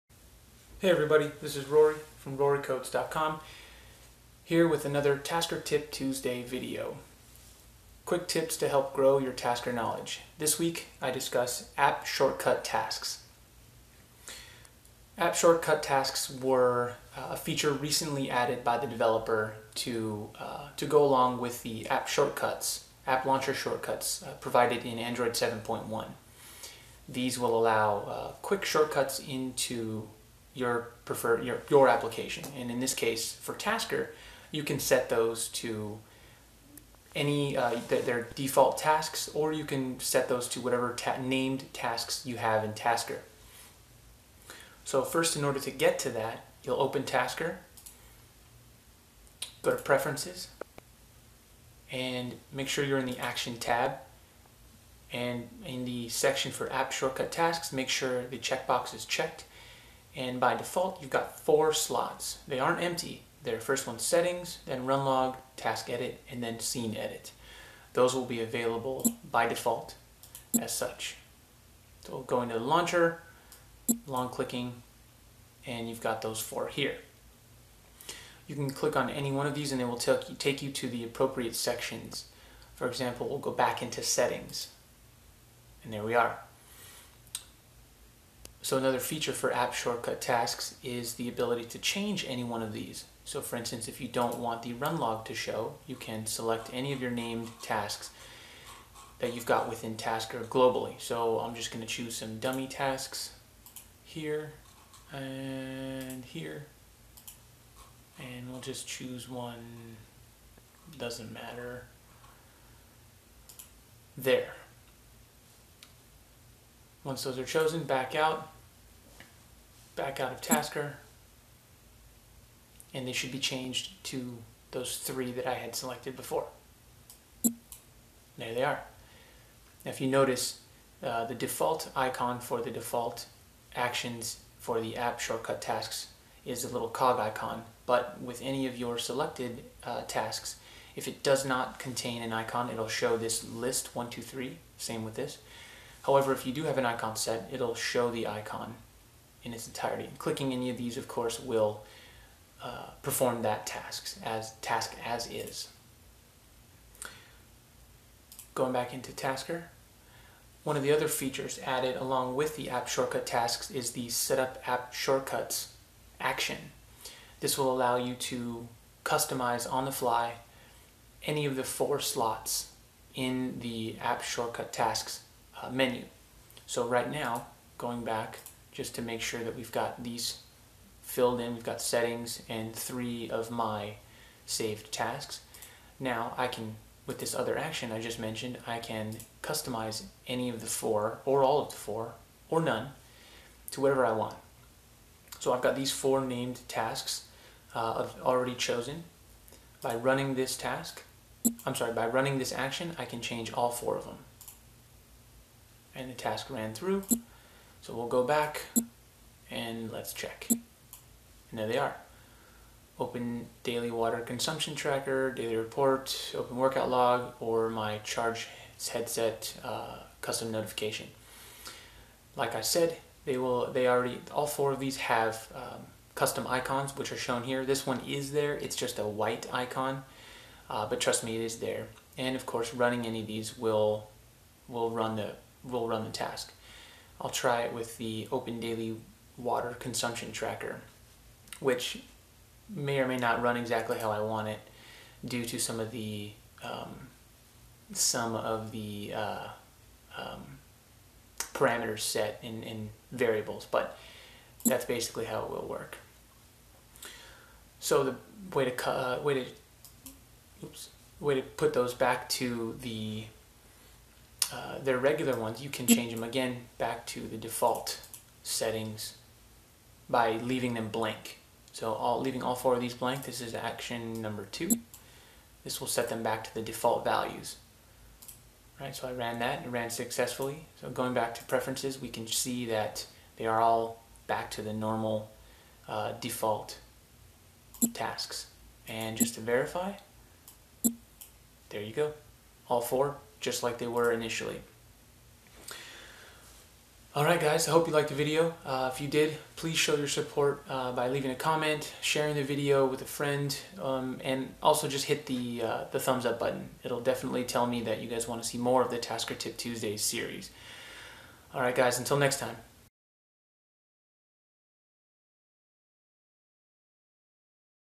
Tasker Tip Tuesdays - How To Add and Customize App Shortcut Tasks
Welcome to Tasker Tip Tuesdays​, quick tips to help grow your Tasker knowledge.  This week I discuss how to add and customize App Shortcut Tasks.  If you want to learn how to access particular tasks quickly with app shortcuts, you’ll love this useful tip.

for the full write-up!